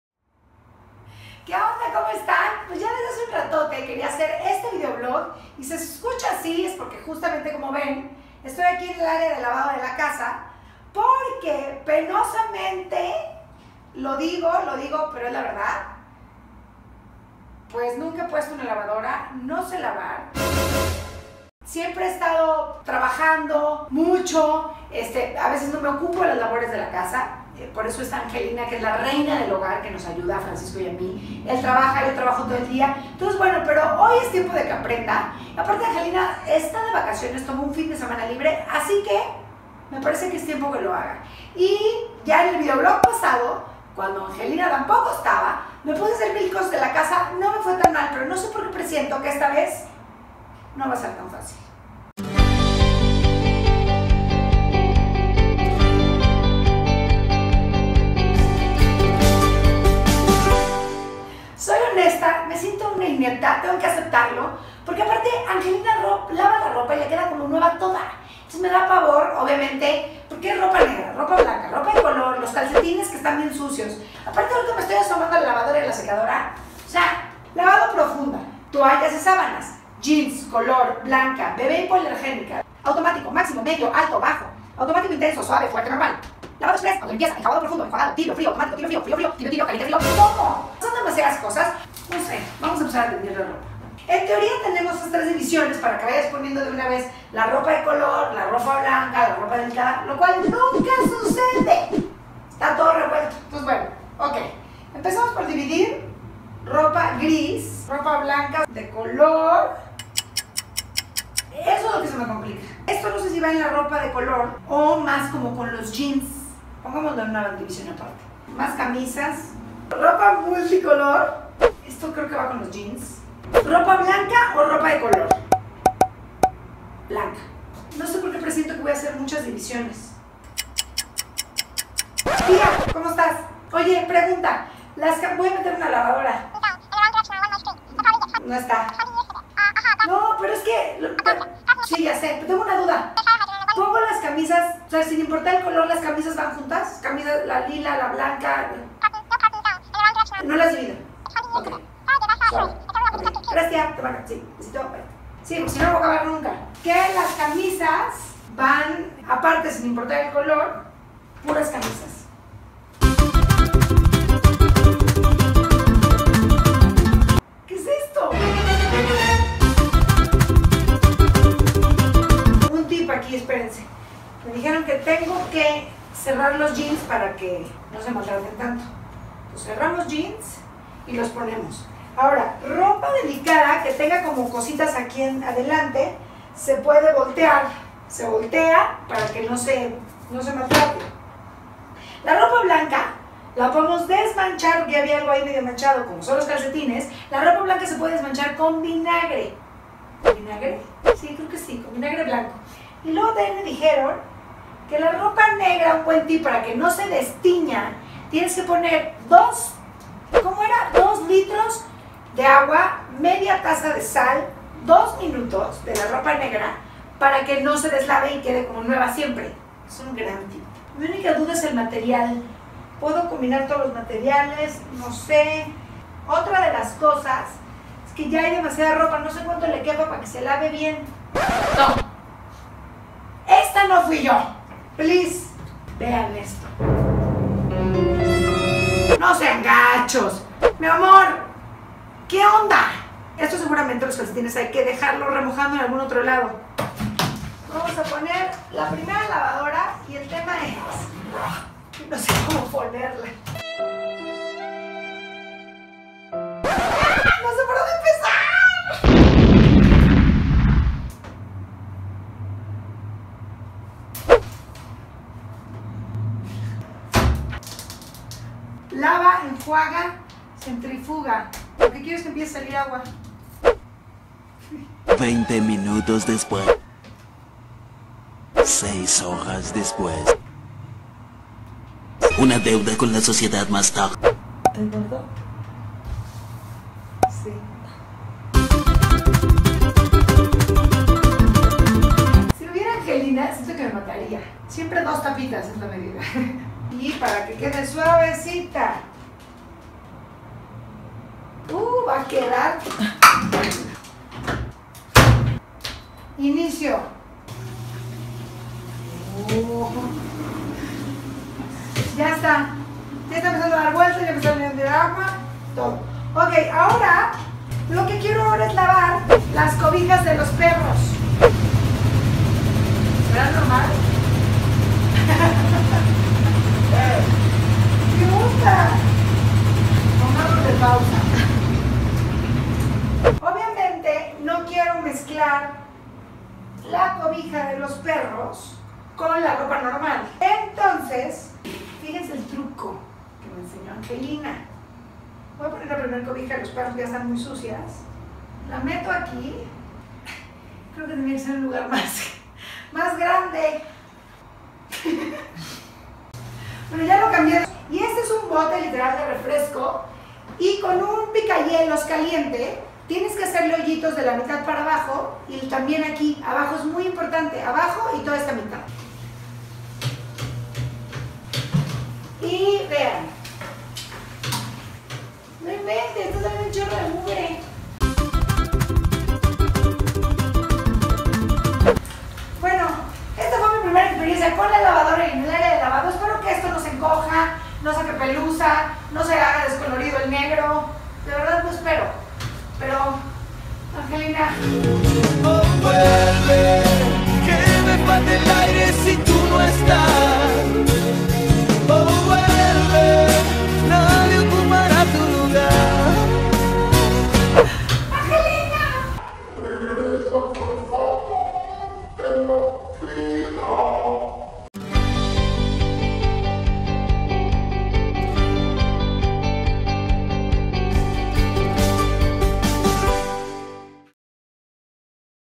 ¡NO VAS A CREER CÓMO HICE UNA LAVADORA MANUAL MUY EFECTIVA! - ERIKA ZABA
Ahora soy ama de casa y de verdad hay cosas que se ven muy fáciles y yo estoy que me rompo la cabeza. Admiro mucho a la mujer mexicana que le da tiempo de hacer todo y bien hecho . mira lo que me pasó en el intento .